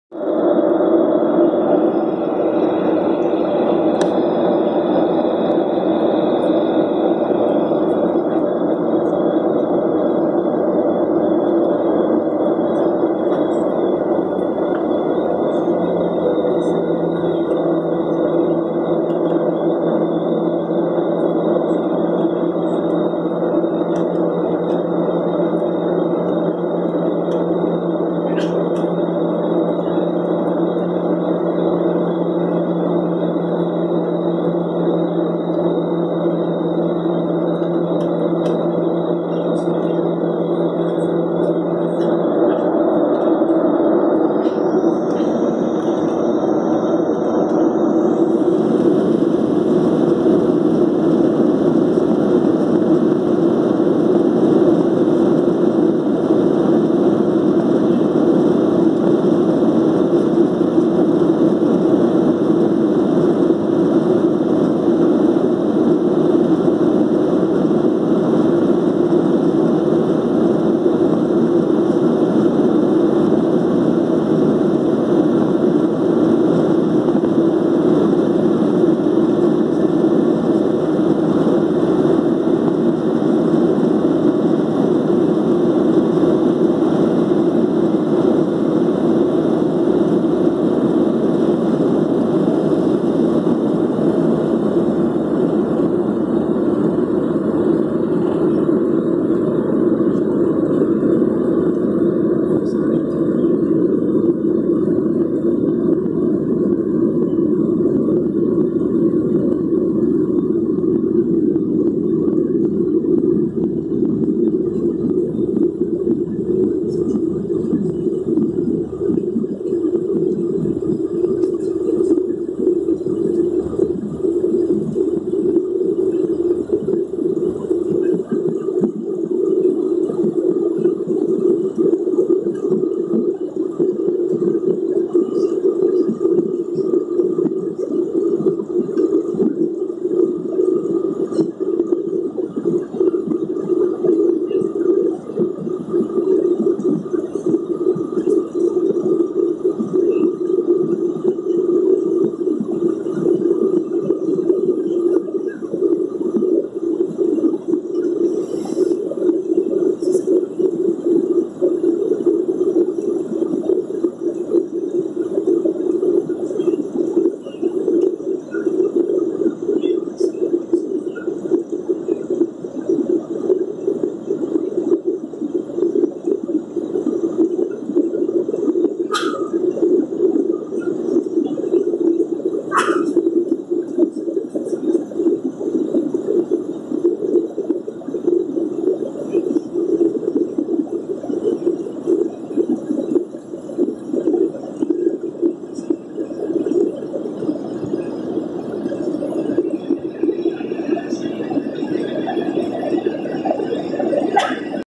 Qantas flight 467 Sydney to Melbourne. Sunset departure.
Boeing 737-800 taken from seat 21A. Runway 16R. Keep a look out for the parallel landing on runway 16L.
You'll see the entrance to Botony Bay just after rotation.
Apologies about the sound... at times sounds a bit like it's under water.